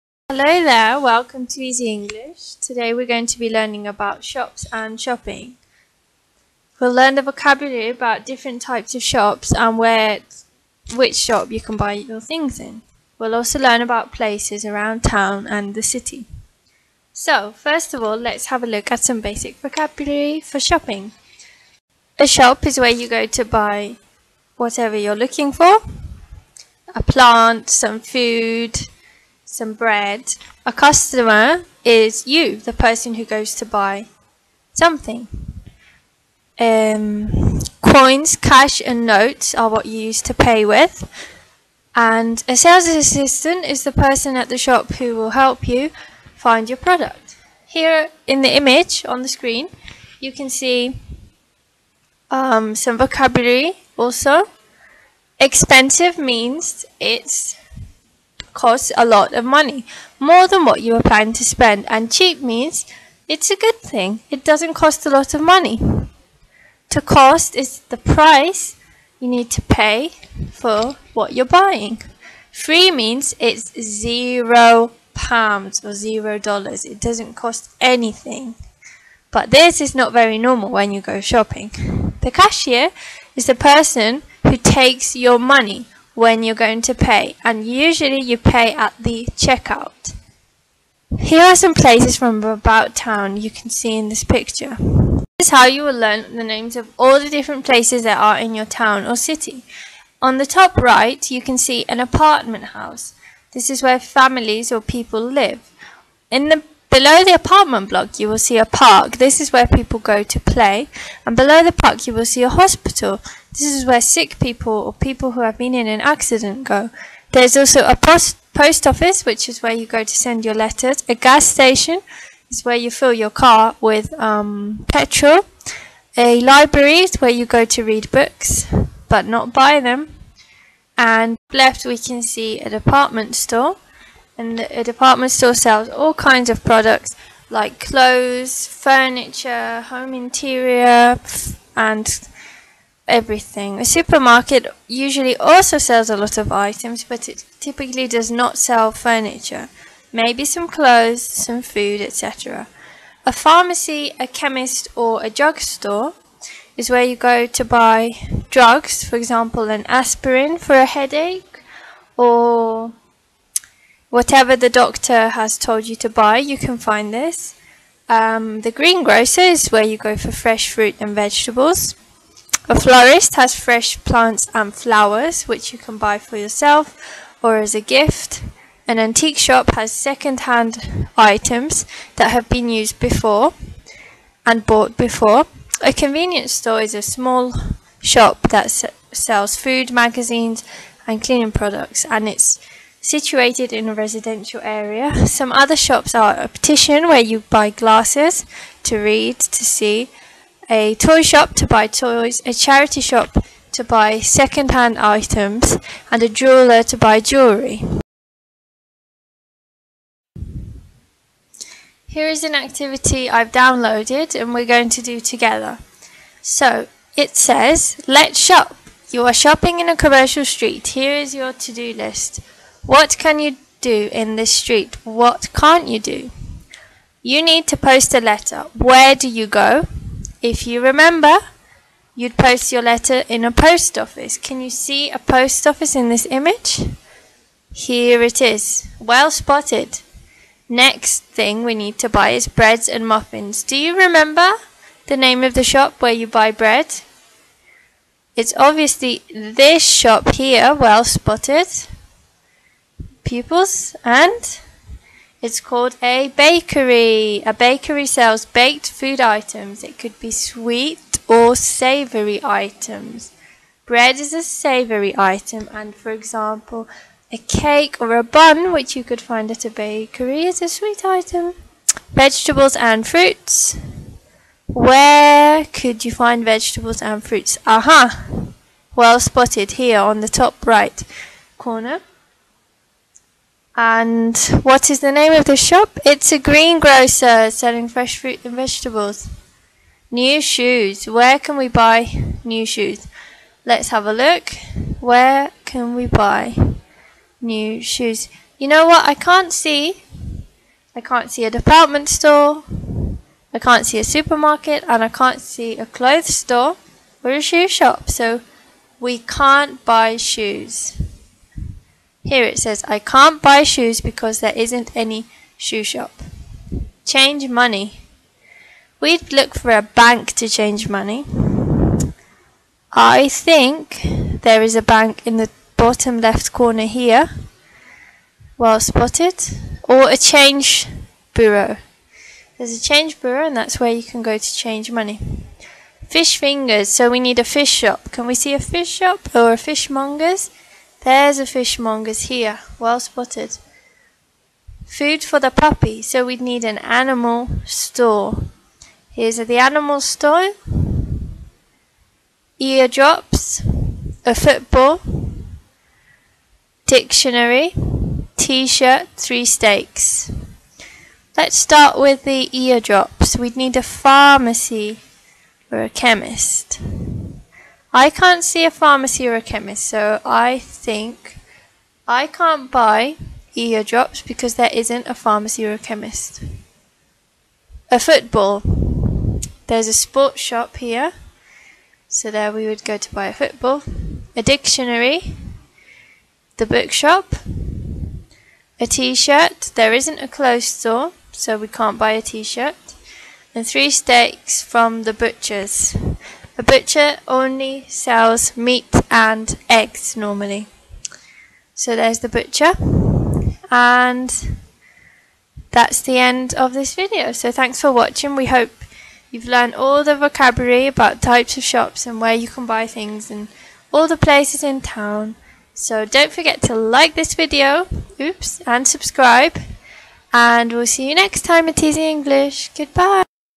English Vocabulary Lesson : Shopping and Types of Shops
Find out vocabulary in English about different shops, shopping and about places around Town or the City